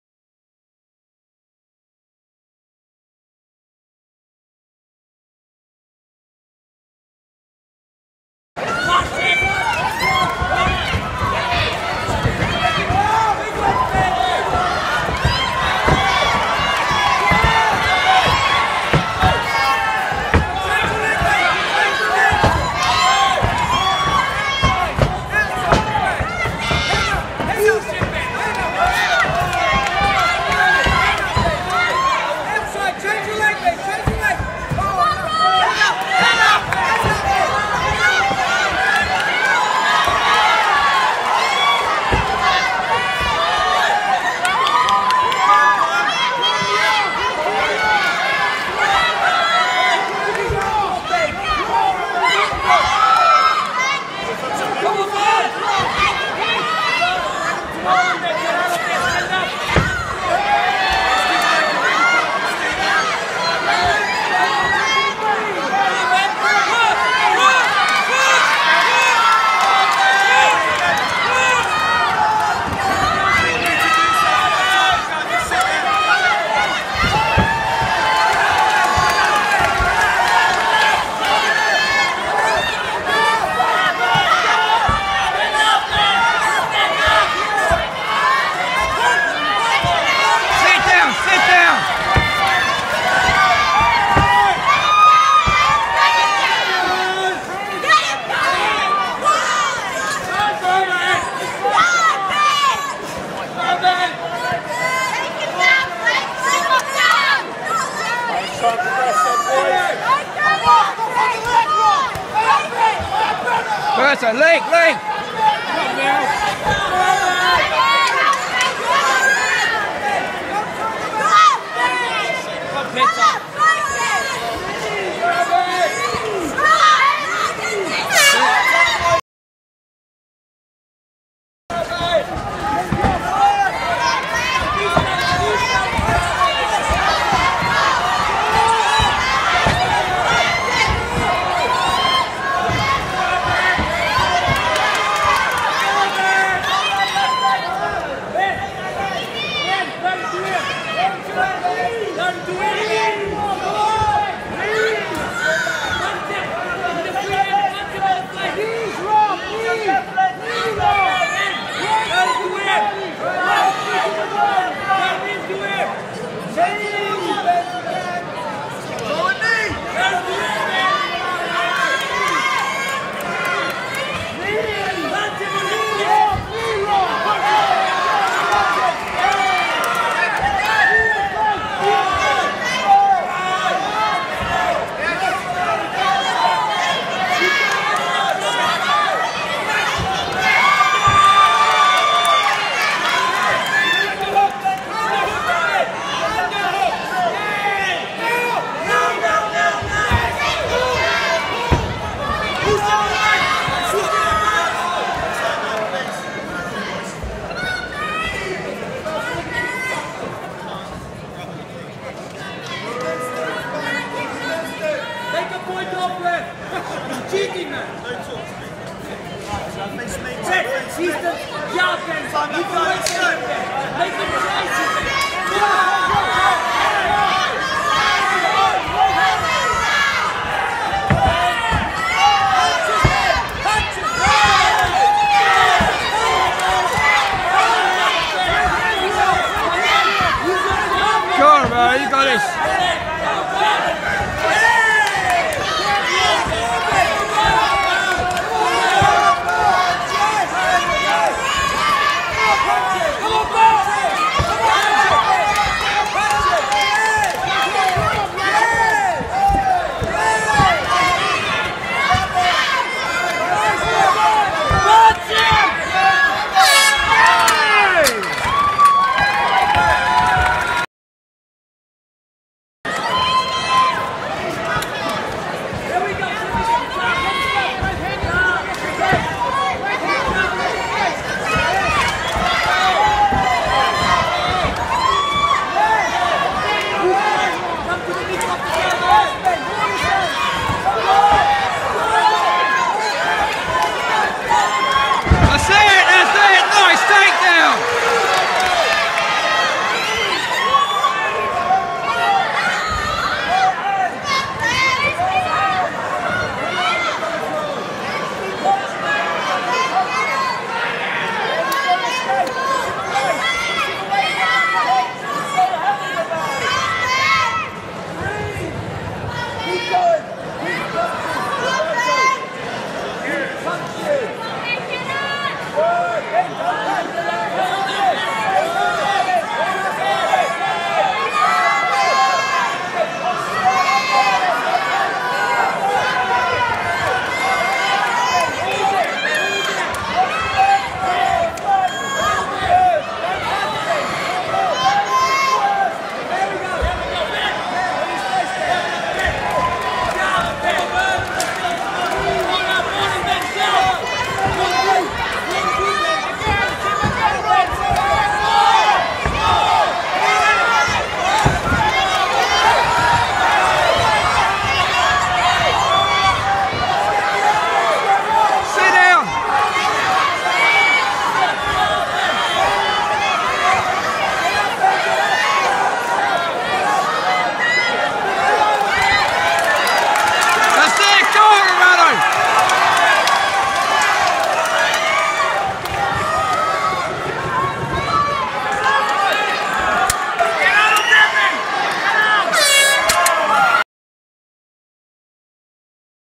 Rumble on the Rock 15 Avalanche Pita v France
White collar match up between Roberto Pita and Ben France. First time fighters in the cage. Held at Fort Regent on the 2nd December 2017.